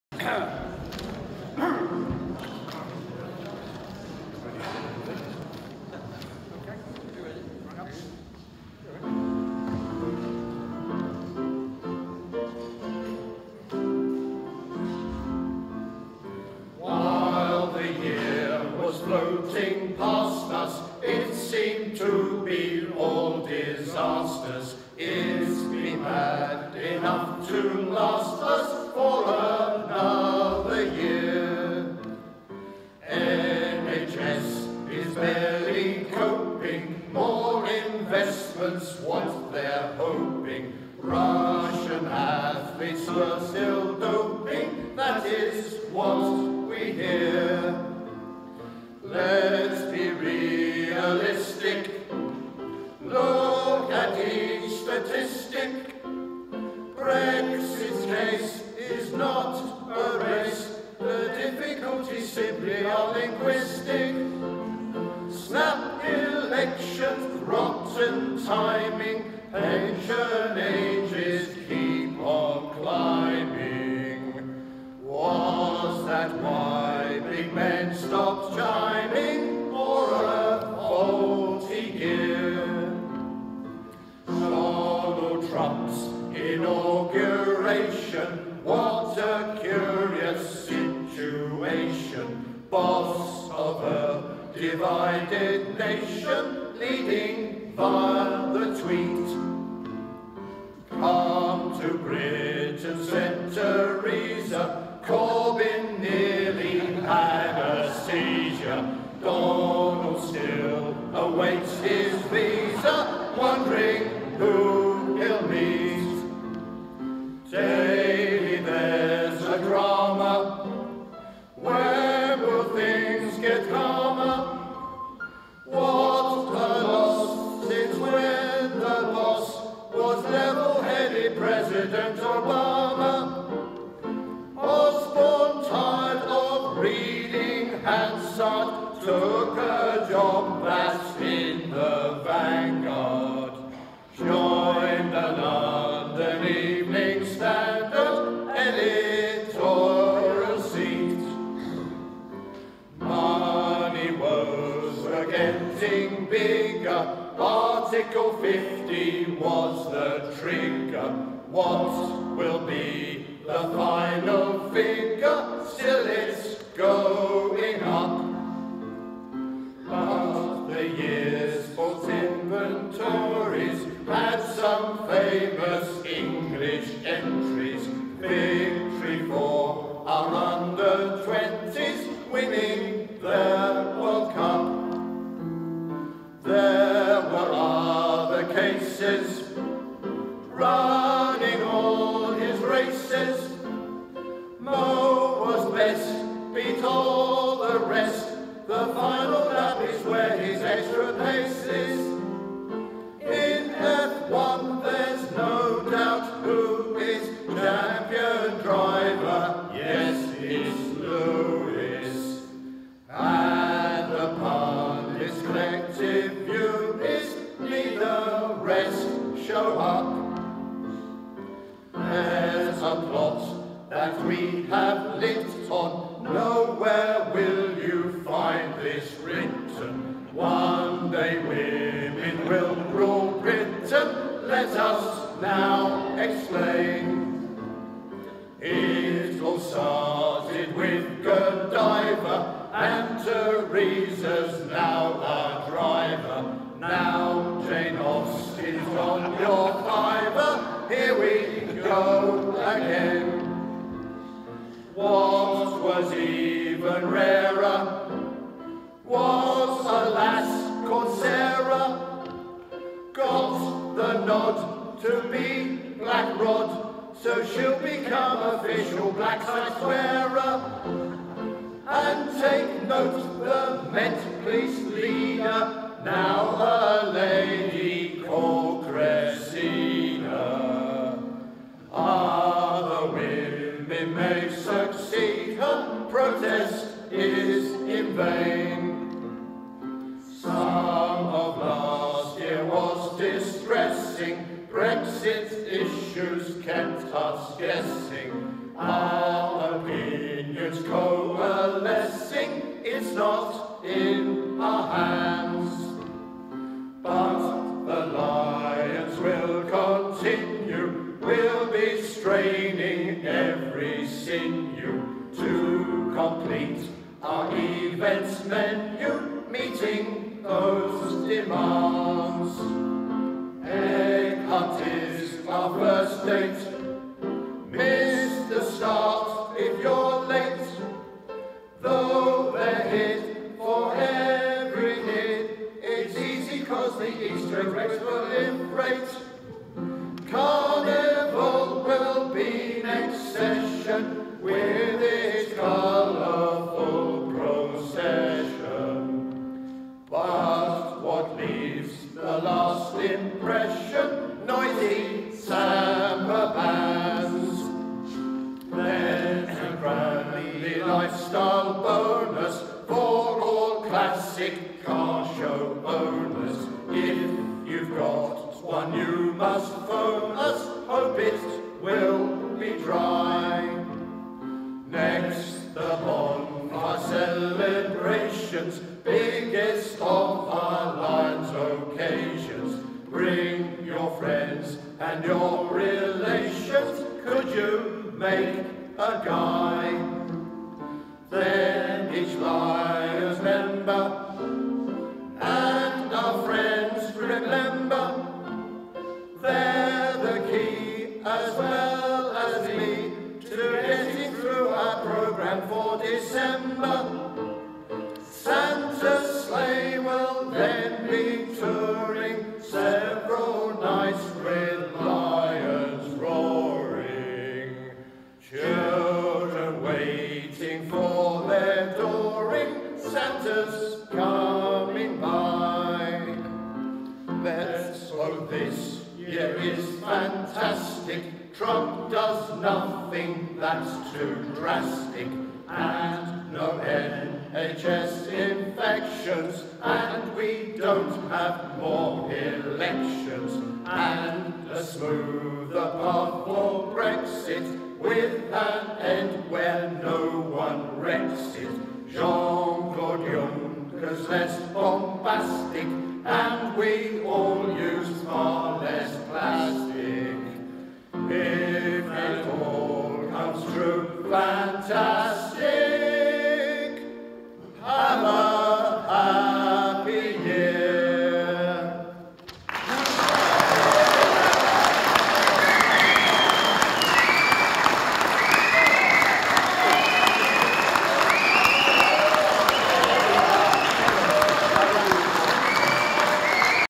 Cranleigh Lions Male Voice Choir 2018
An entertaining performance at the Cranleigh Lions Presentation Evening on Thursday 15th February 2018.